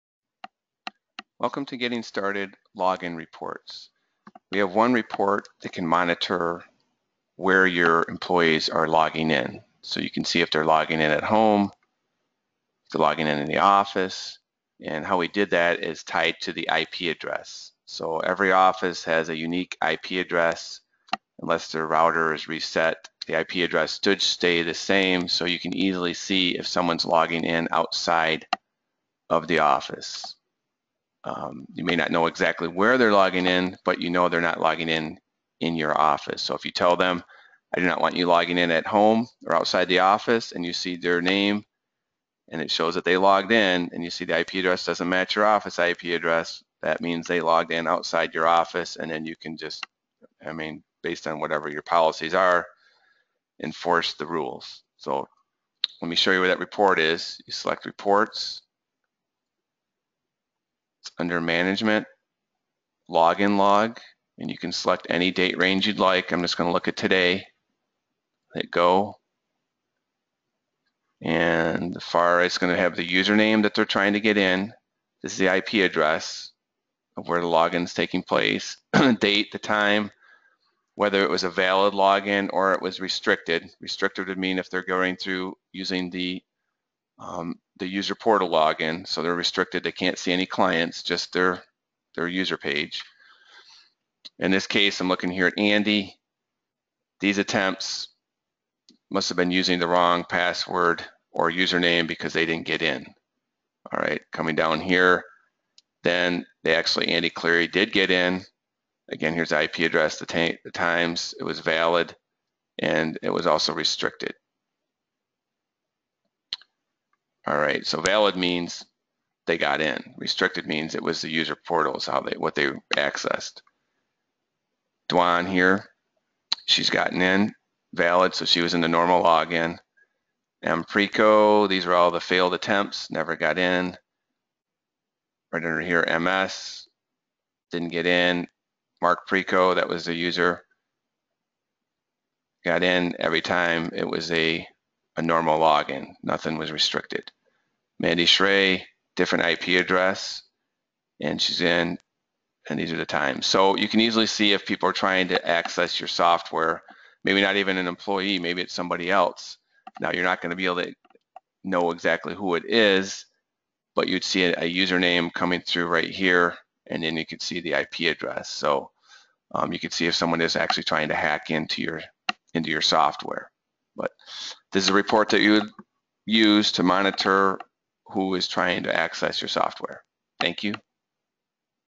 Getting Started - Login Reports
This video explains how to use reports to monitor who is, successfully or unsuccessfully, login in to your ATOM Software.